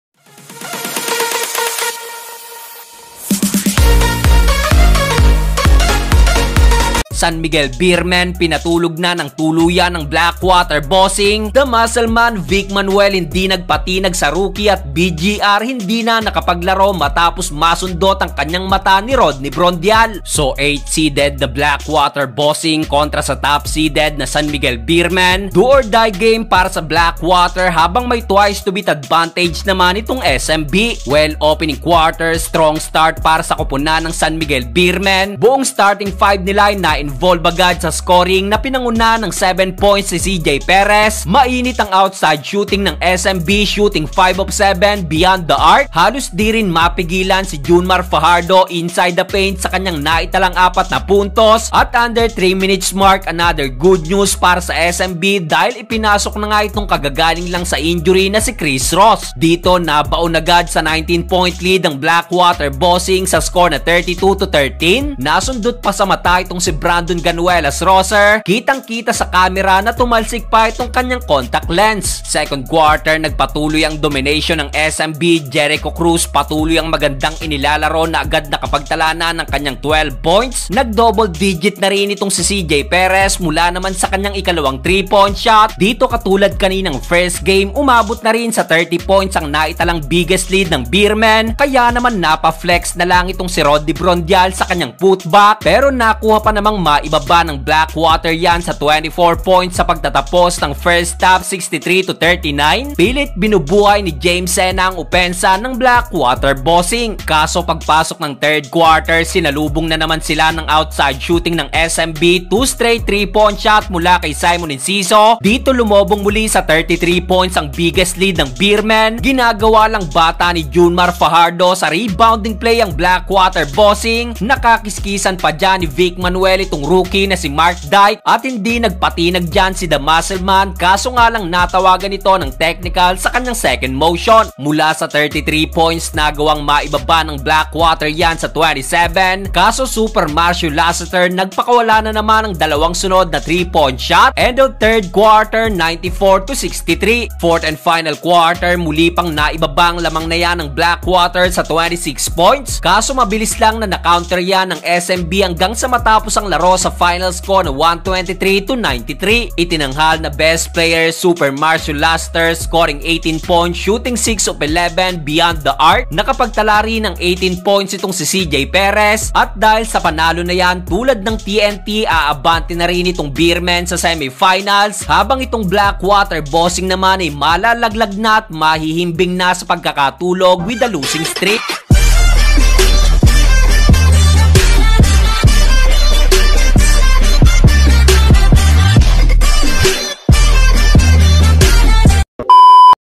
Vic manuel DI NAGPASINDAK SA ROOKIE _SMB tuluyan nang pinatulog Ang black water bossing vlog 130
FAKE pogi tv vic manuel hindi nagpasindak sa ROOKIE _ smb tuluyan nang pinatulog Ang black water July 29,2022 vlog 130